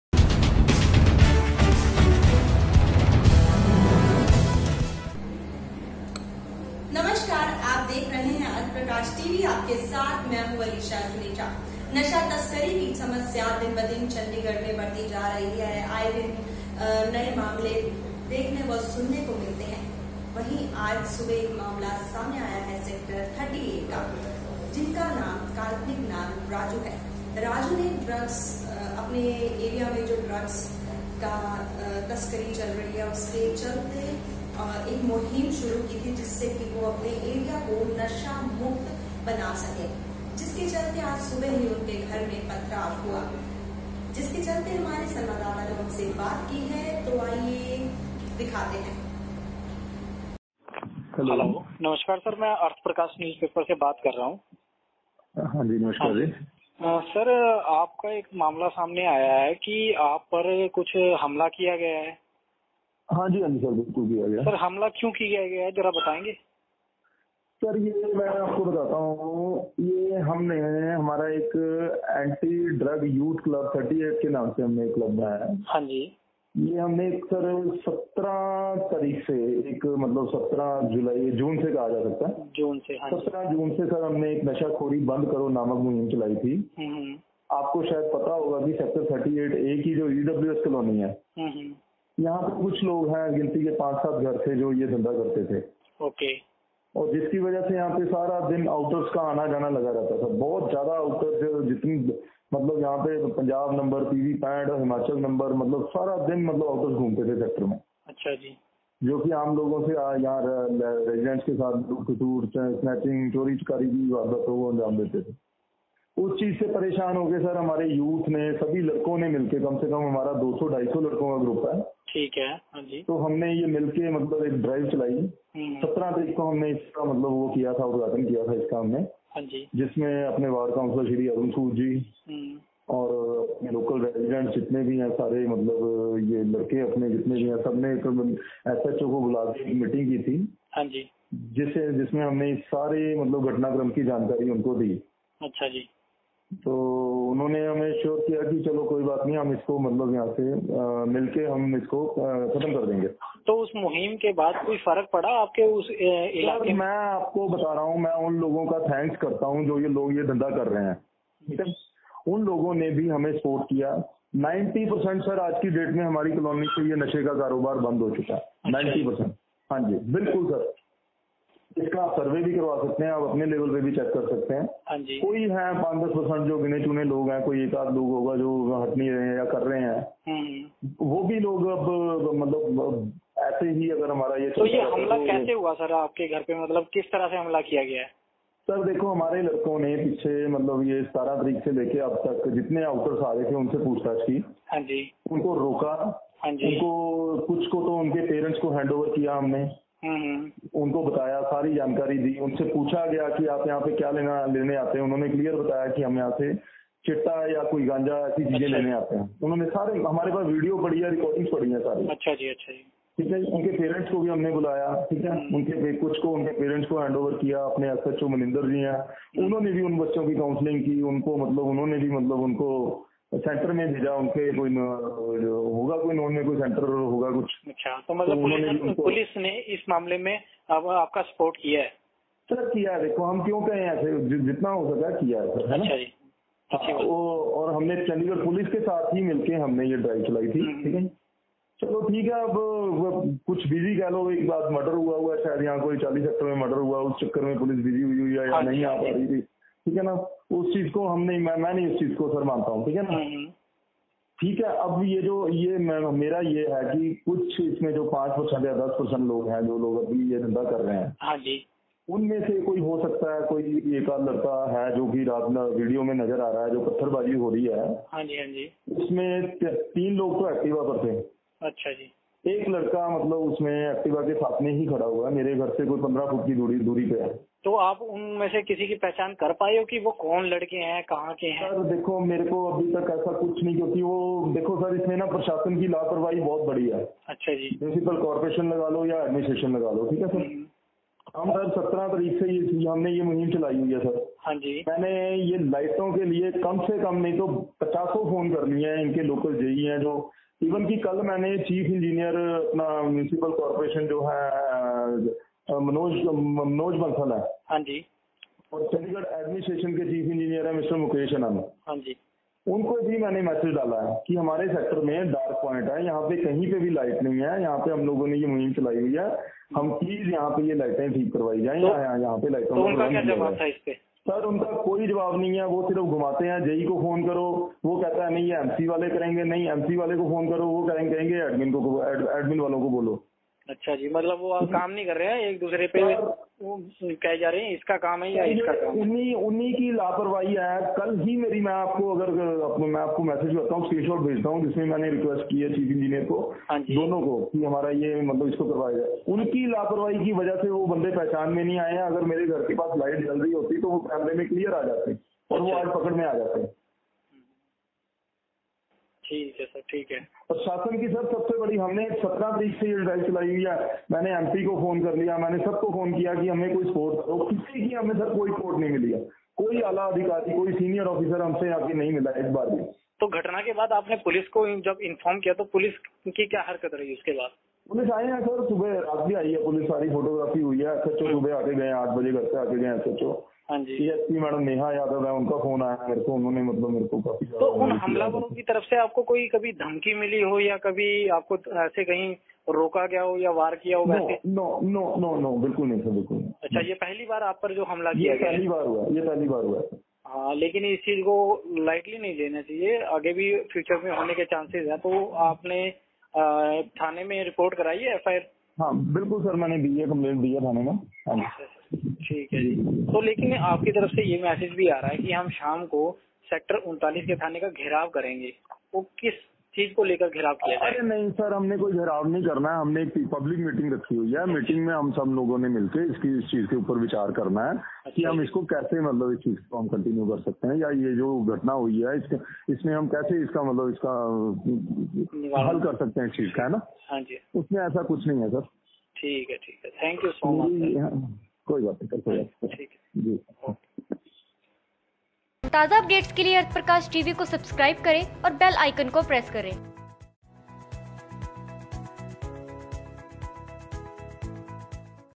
चंडीगढ़ : नशे के खिलाफ आवाज़ उठाने वाले के घर किसने की पत्थरबाज़ी सुनिए पीड़ित की जुबानी
चंडीगढ़ में नशे के खिलाफ आवाज़ उठाने वाले के घर किसने की पत्थरबाज़ी सुनिए पीड़ित की जुबानी